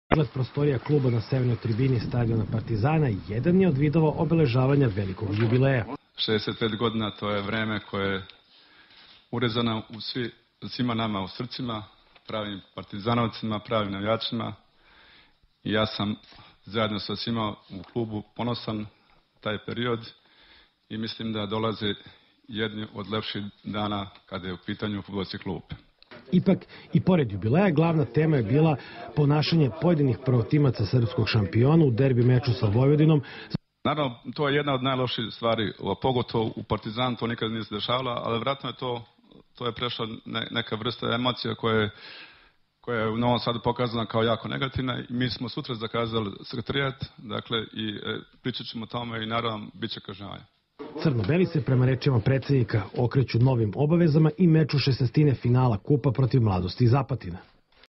65 godina JSD Partizan
Jugoslovensko sportsko društvo Partizan danas slavi šezdeset pet godina
postojanja. JSD Partizan spada u red najveæih sportskih društva u svetu, a
po rezultatima, ostvarenjima i broju svojih vrhunskih sportista tokom
proteklih 65 godina spada u red najuspešnijih na svetu.